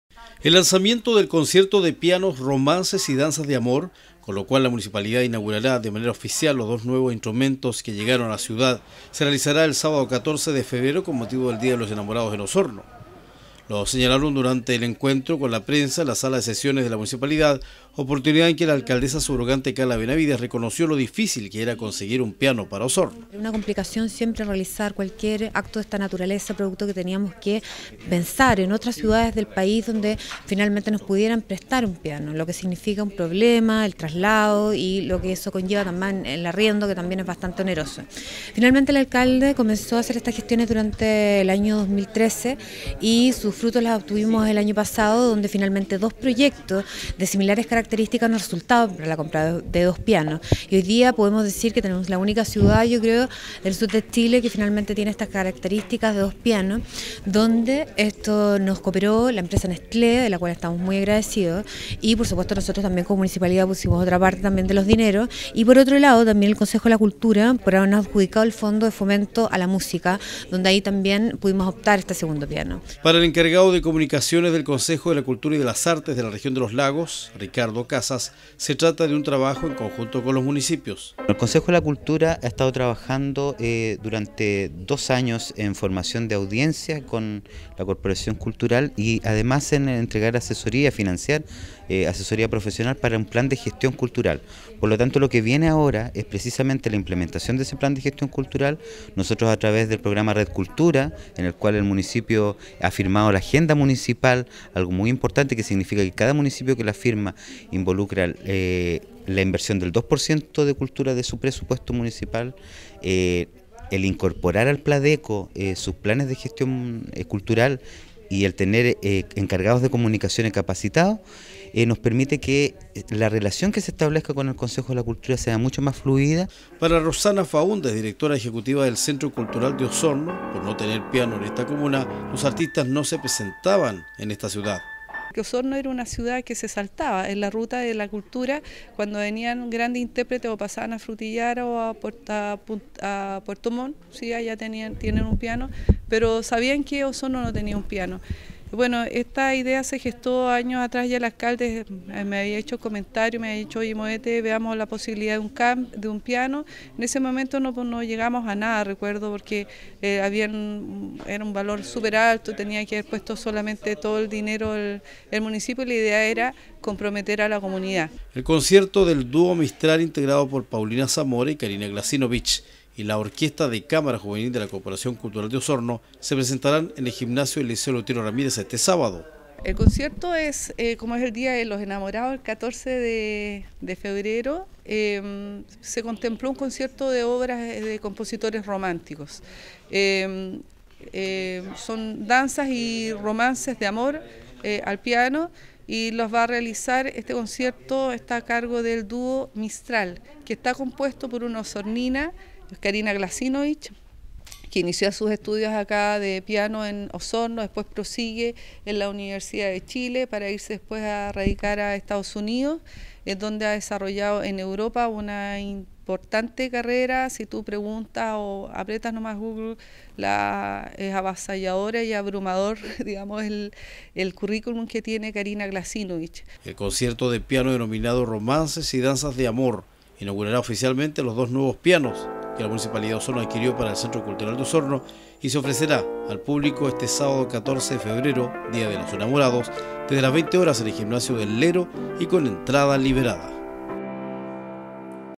"Romances y Danzas de Amor" en Día de San Valentín , en Osorno.
Un concierto denominado "Romances y Danzas de Amor" inaugurará oficialmente dos nuevos pianos que la municipalidad adquirió para el Centro Cultural de Osorno.